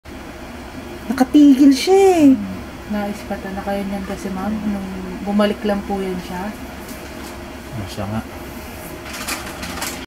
Gone in 10 seconds
My bag being snatched along Taft Avenue near Buendia. Wasn't really thinking that the guy was taking my bag. I really thought sumabit lang pag daan nya.